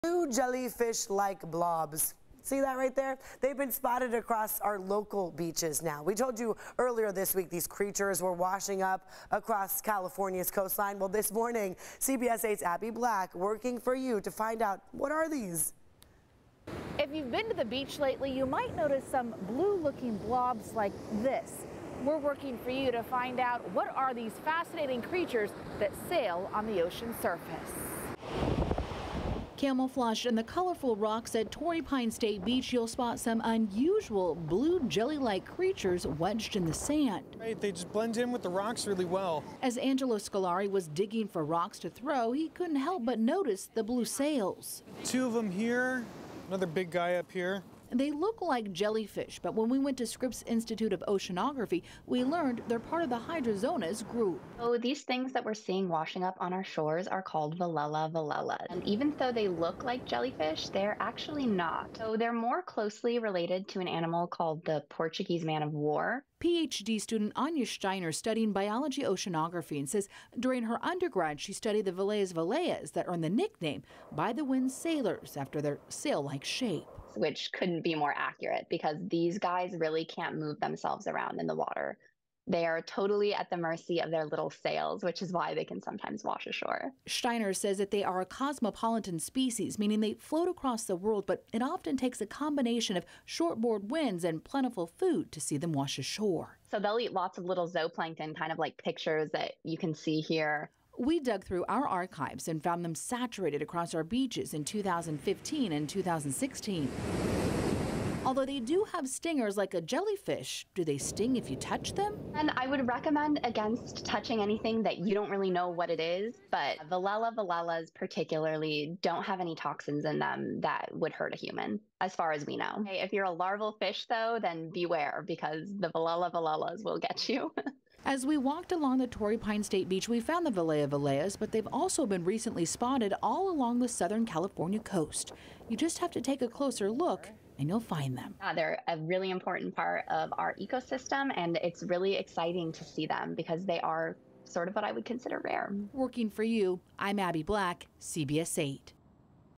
What are the blue looking blobs on San Diego's beaches?
If you've been to the beach lately, you may have noticed blue blobs scattered across the sand. We're working for you to learn more about these fascinating sea creatures that sail on the ocean's surface.

“They're a really important part of our ecosystem. And it's really exciting to see them because they are sort of what I would consider rare,” said Anya Stanjer, PhD student at Scripps Institution of Oceanography.